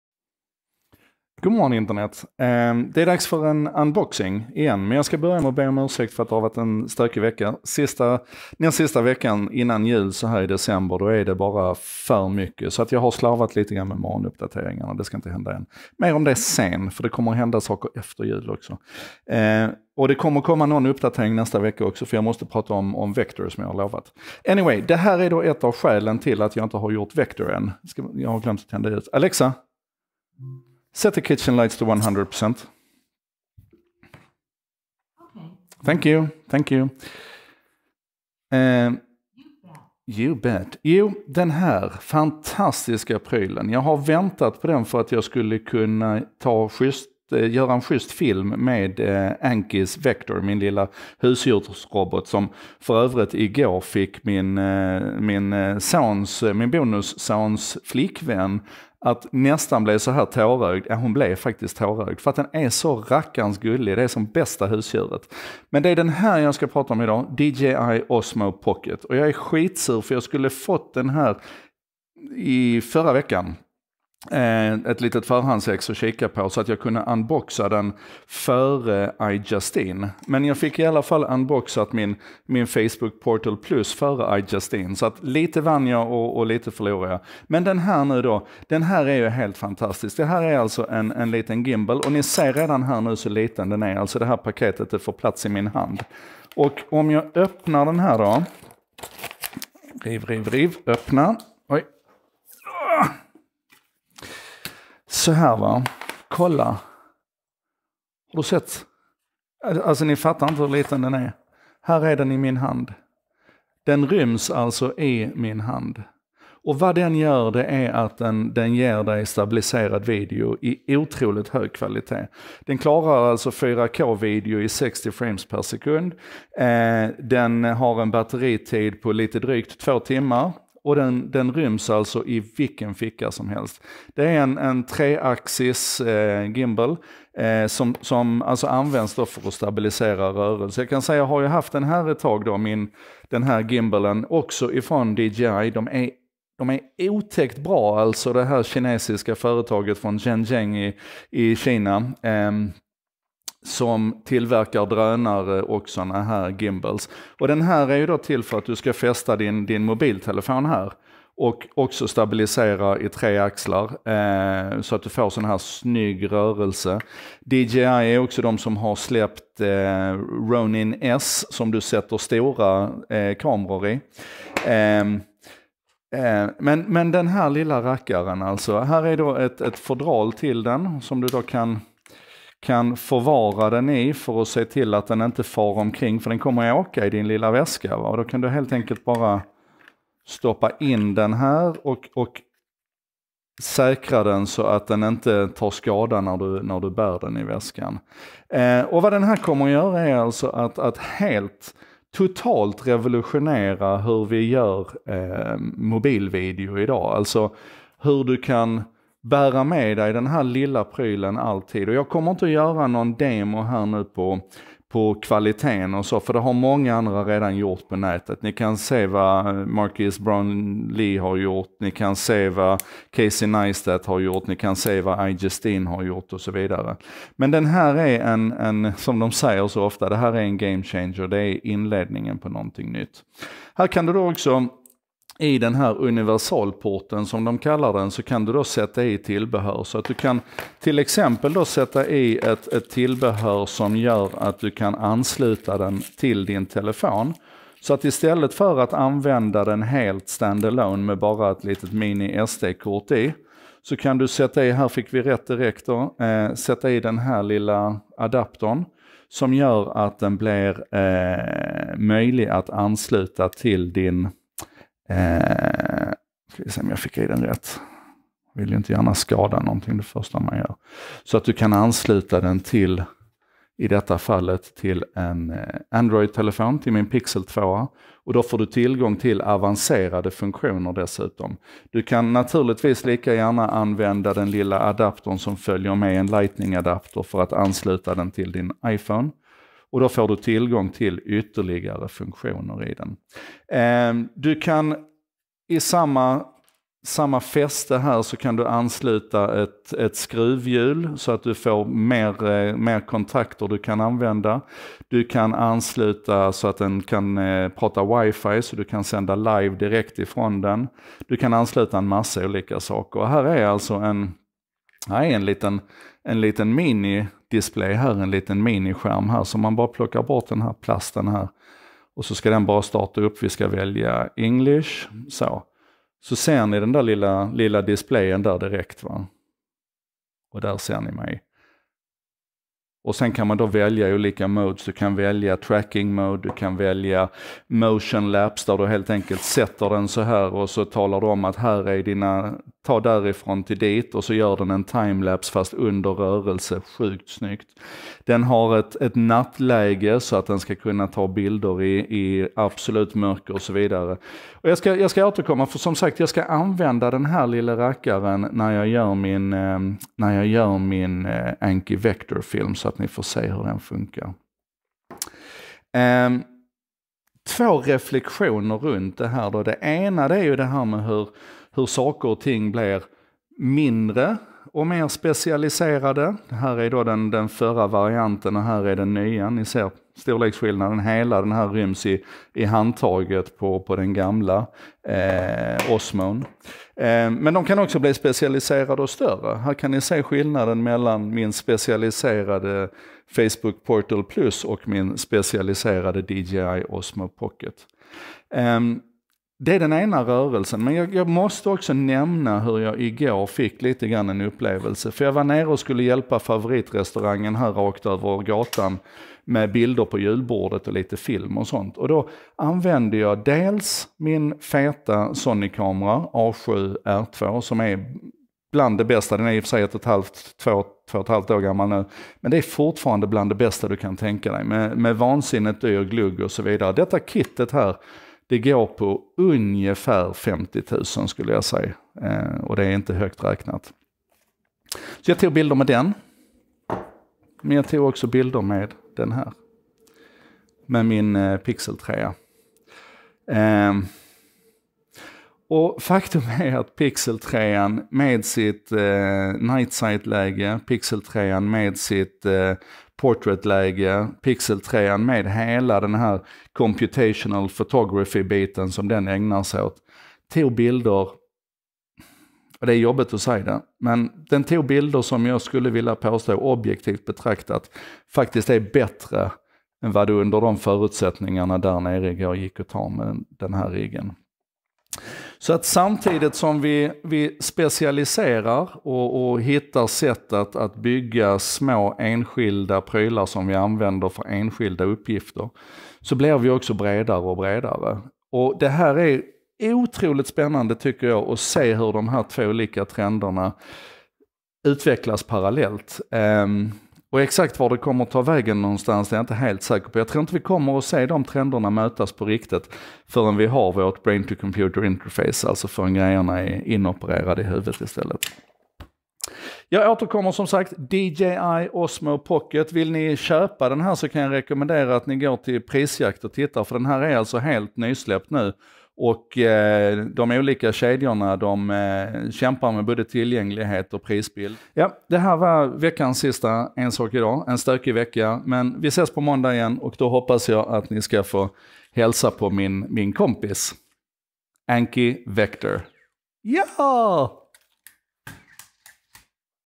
282. Unboxing: DJI Osmo Pocket - en gamechanger
282. Unboxing: DJI Osmo Pocket - en gamechanger
Ingenjörskonst på hög nivå, igen, från DJI. Att en enhet som ryms i halva min hand ska kunna leverera den här kvaliteten är helt otroligt. Det är magiskt i sig själv, men samtidigt är det ännu mer intressant vad det säger om vägen framåt.